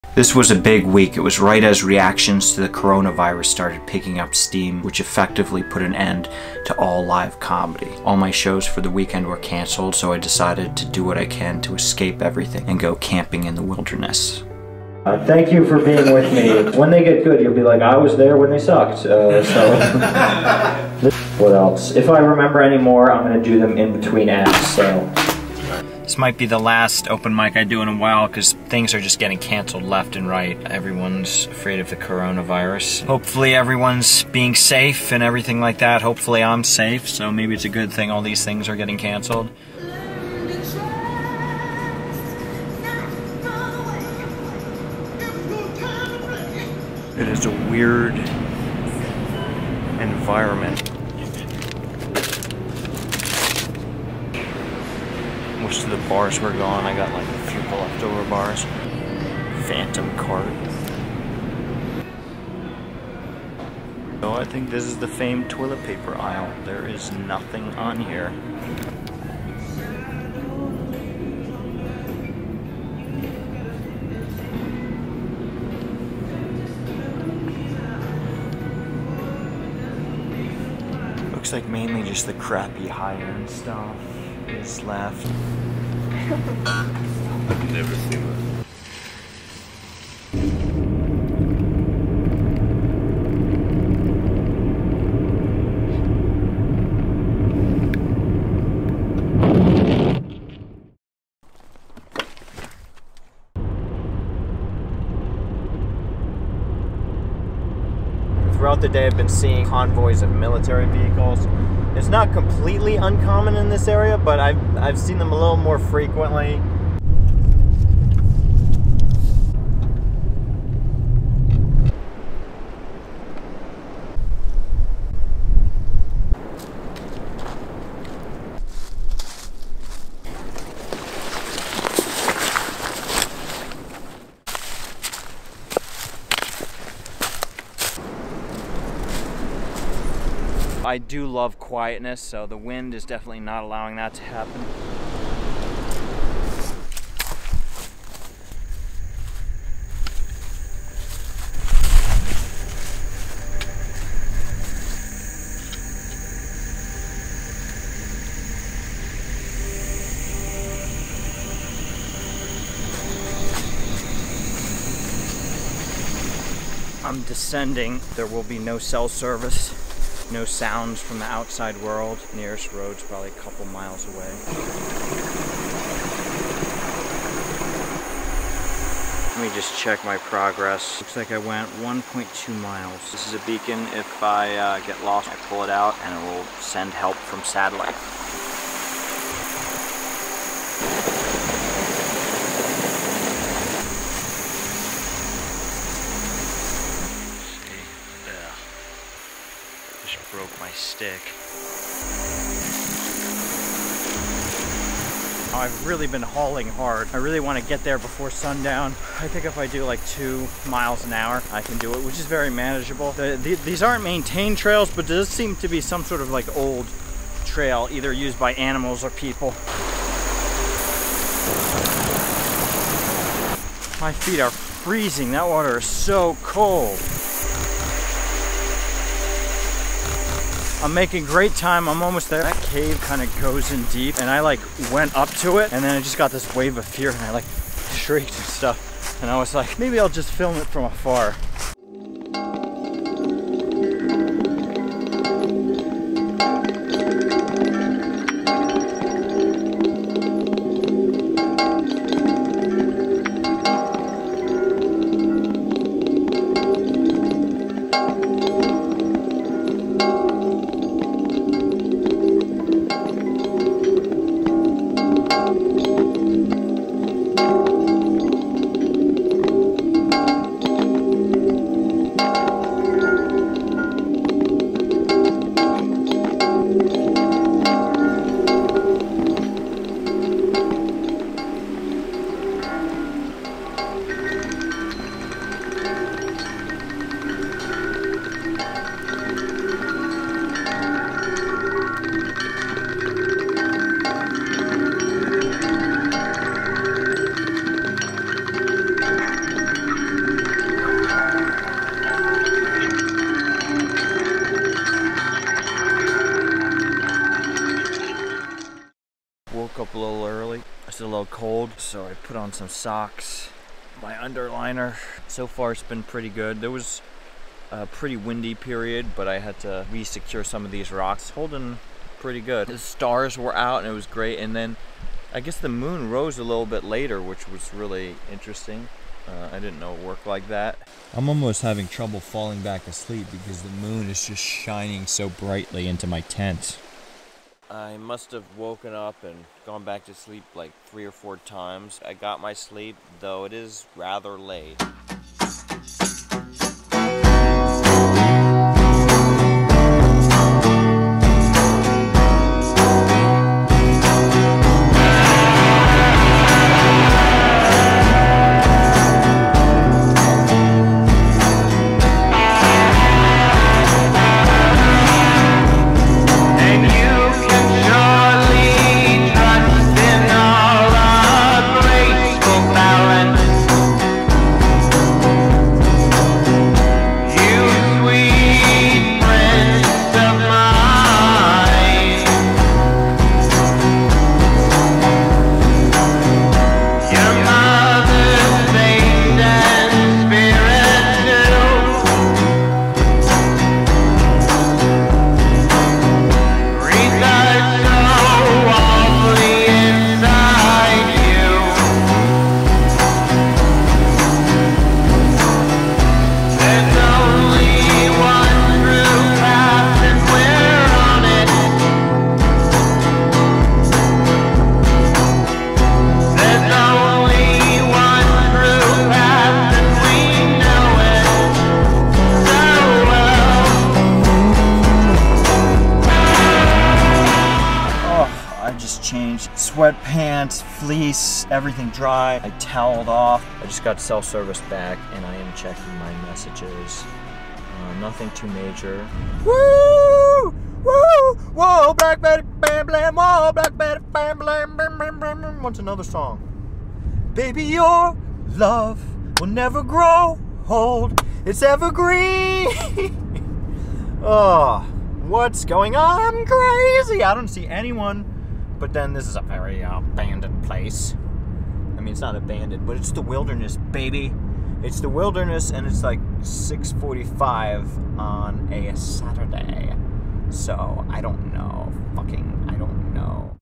I Record the Weekend the Lockdown Shut Down Comedy
A web docuseries about a Stand-up Comedian trying to make it in Comedy. This episode I perform my last stand up set for a while as a deadly pandemic shuts down comedy as we know it. To calm down I decided to escape society for a couple days.

The Comedy Diaries of James McAliney is created by James McAliney and Andy Eklund. It is shot and edited by James McAliney and produced by James McAliney and Andrew Fox.